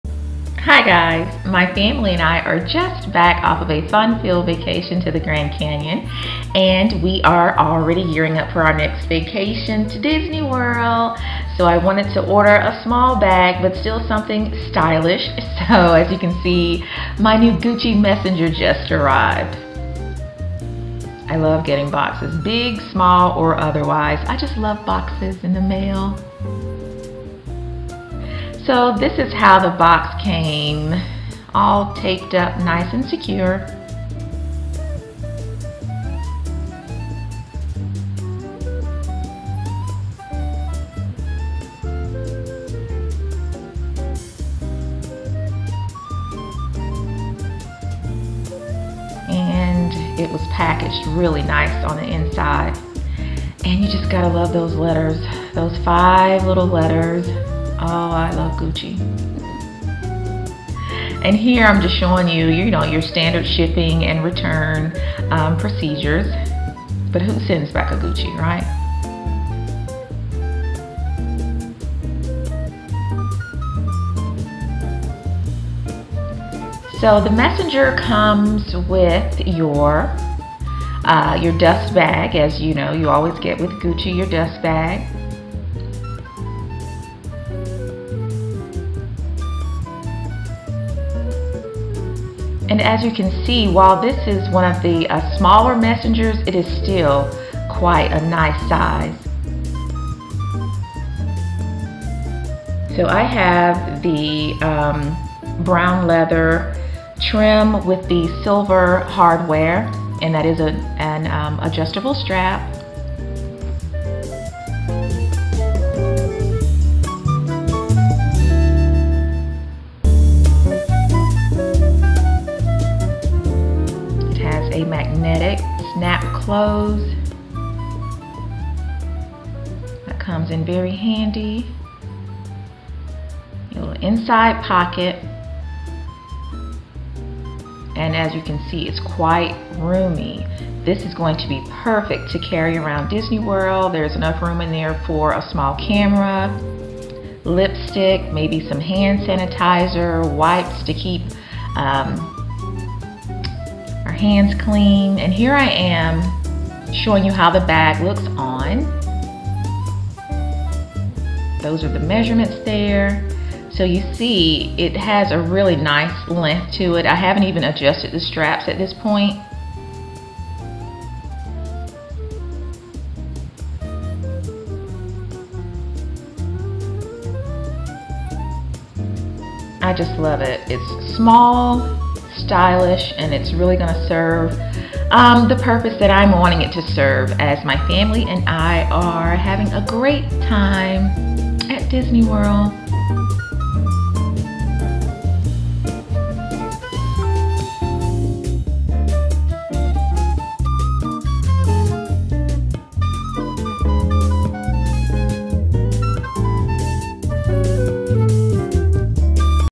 GUCCI Messenger UnBoxing
I needed (ok- WANTED) a new bag for an upcoming vacation to Disney World. So, I ordered the Gucci W & U Lifestyle Messenger Bag. Here's the unboxing...

Don't Forget To View In HD!!!